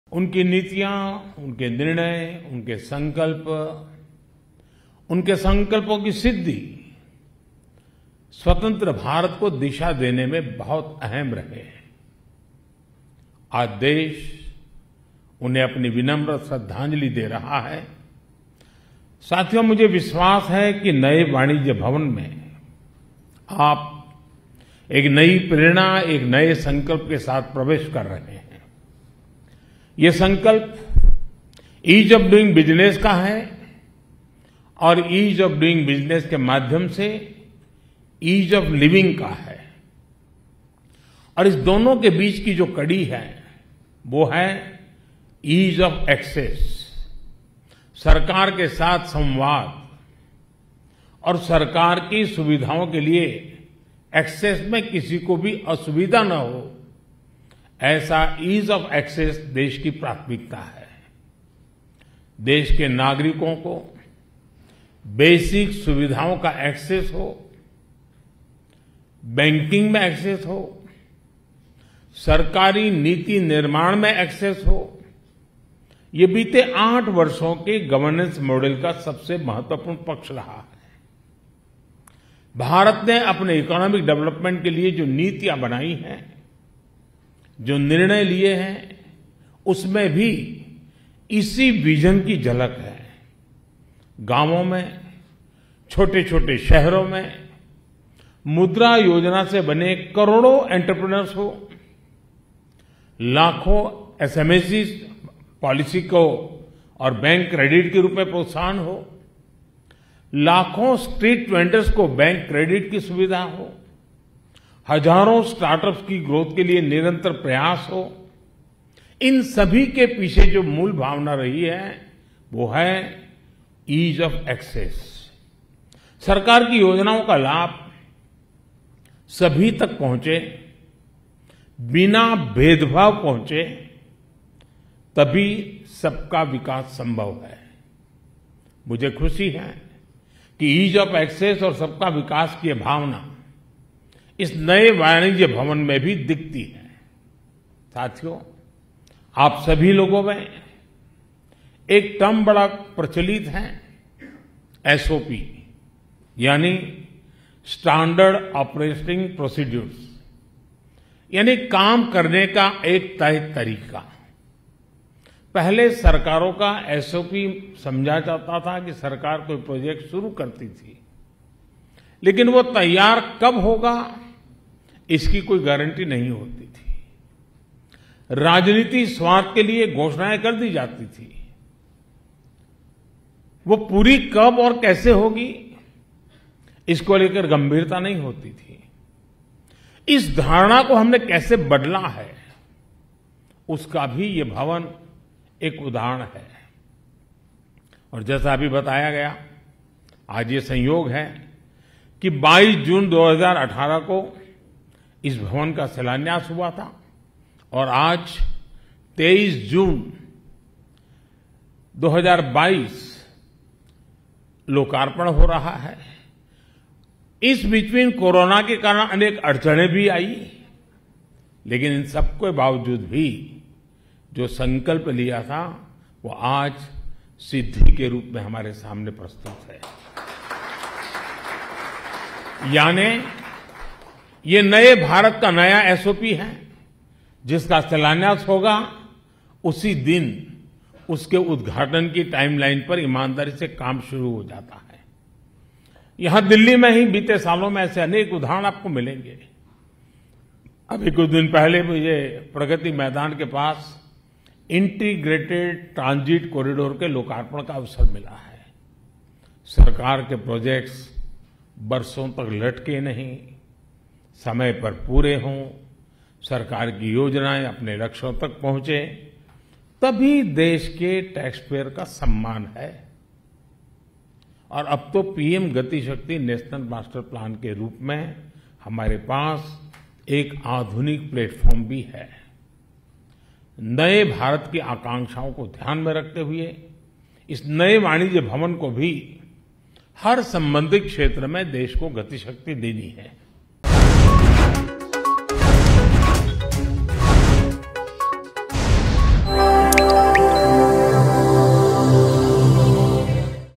PM Narendra Modi inaugurated 'Vanijya Bhawan' in Delhi
Prime Minister Narendra Modi inaugurated 'Vanijya Bhawan' in Delhi.